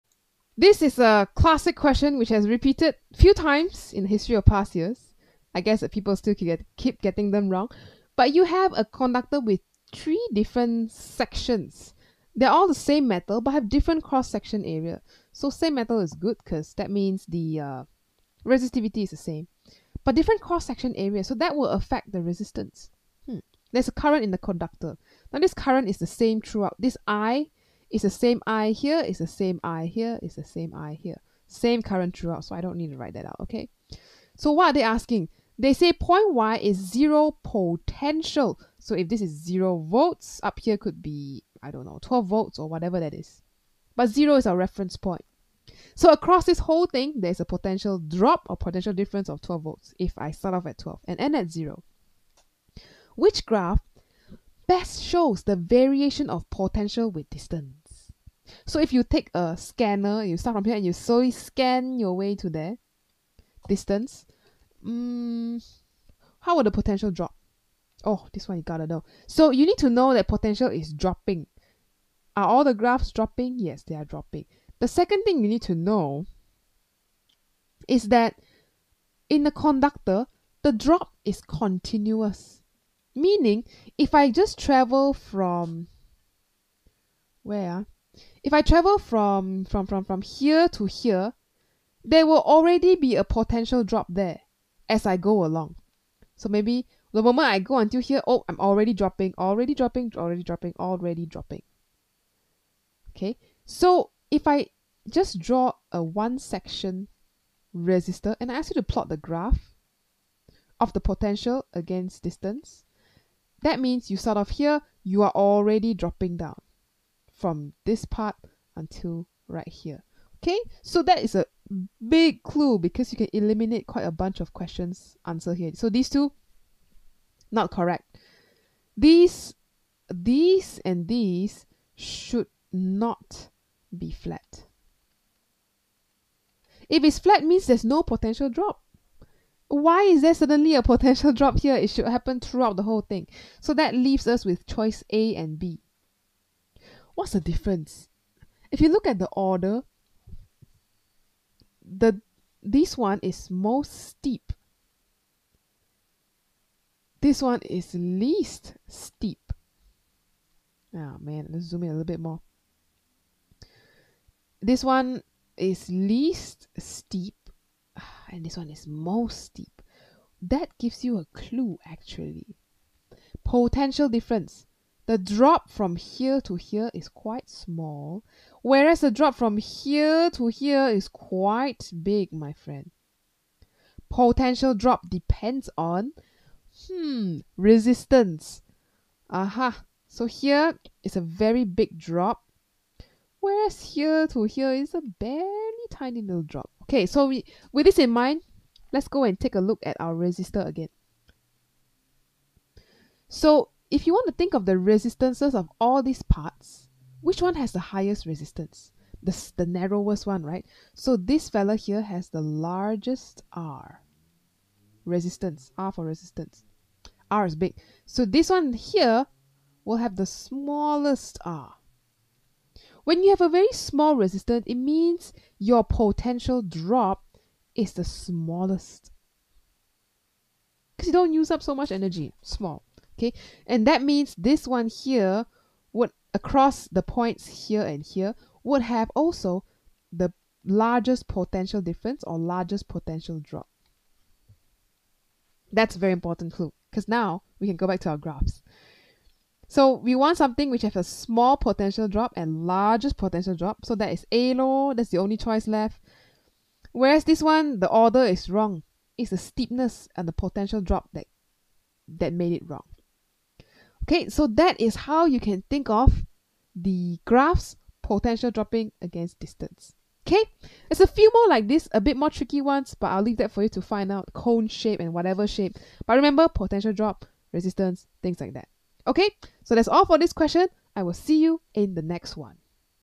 MJ20 P12 Q35 / ON14 P11 Q33 Potential Drop Graph | CAIE A Level 9702 Physics
9702/12/M/J/20: A conductor consists of three wires connected in series. The wires are all made of the same metal but have different cross-sectional areas. There is a current I in the conductor.

Point Y on the conductor is at zero potential. Which graph best shows the variation of potential V with distance along the conductor?
(Also 9702/11/O/N/14)

9702_s20_qp12
9702_w14_qp11#CambridgeALevelPhysics